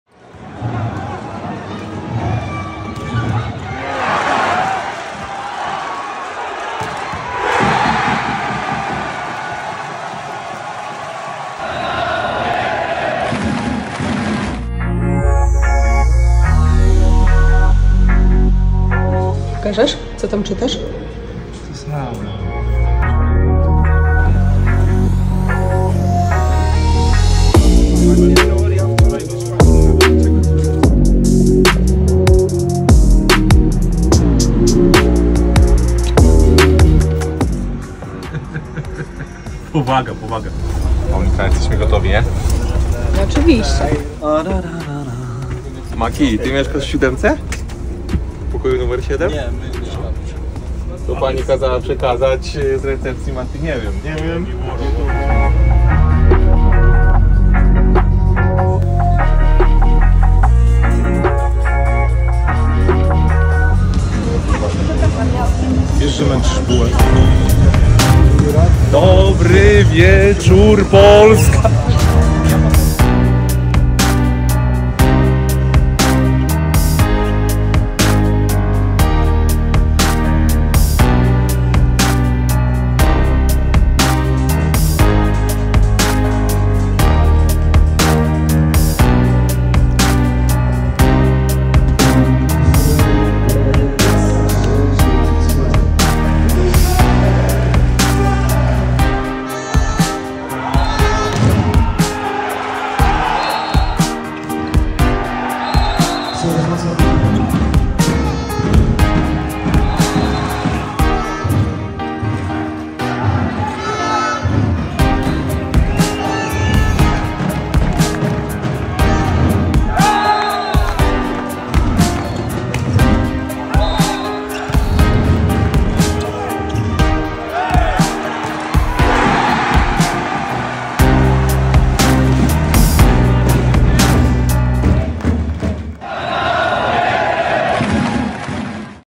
Zabrakło dwóch punktów, w niedzielę ćwierćfinał nr 4
Po bardzo trudnym meczu Jurajscy Rycerze ulegli w Iławie Indykpolowi AZS-owi Olsztyn 2:3. Po dobrych zmianach Dawida Dulskiego i Marcina Walińskiego nasz zespół doprowadził do tie-breaka, w którym remisował 13:13, ale dwie decydujące piłki padłem łupem gospodarzy. Tym samym w niedzielę o 14:45 odbędzie się czwarte spotkanie ćwierćfinałowe, a Aluron CMC Warta Zawiercie w rywalizacji do trzech zwycięstw prowadzi 2-1.